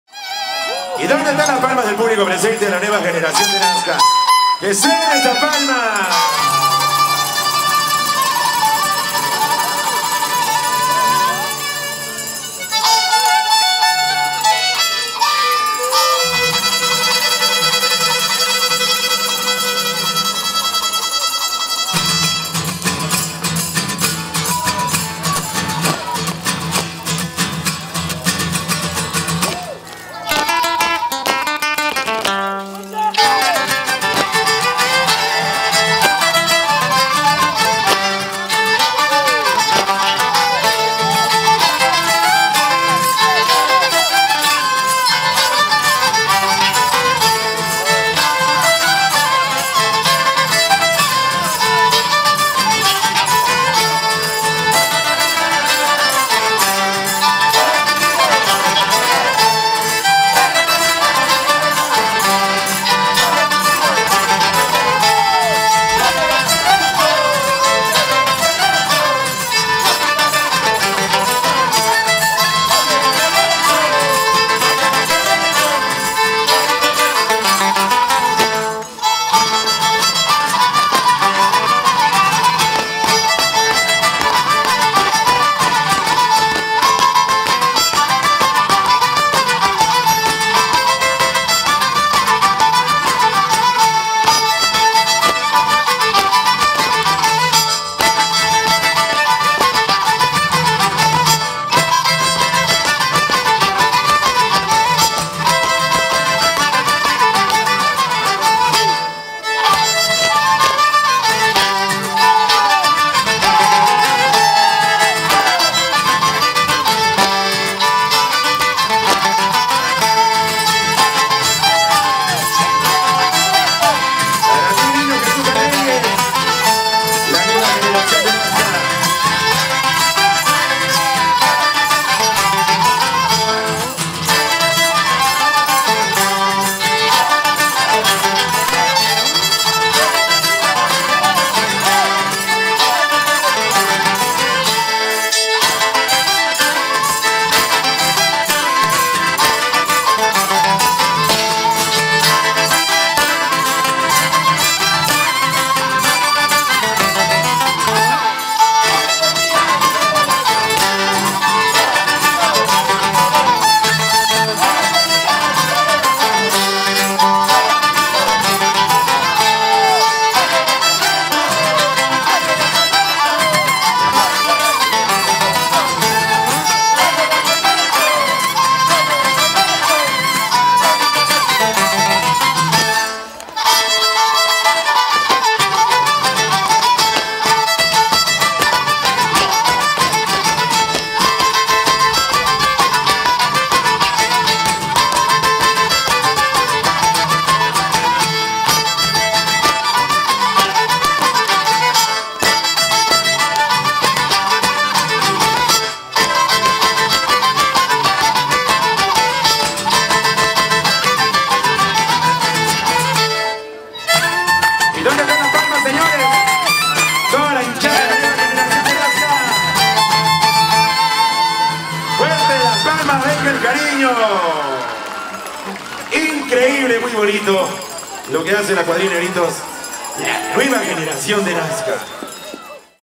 Bajada de Reyes 2020 ► Cuadrilla de Negritos La Nueva Generación ► Segunda presentación
Bajada de Reyes 2020 en Nasca - Ica - Perú.
Bajada tradicional en honor al Niño Jesús de Reyes.
Cuadrilla de Negritos La Nueva Generación de Nasca
SEGUNDA PRESENTACIÓN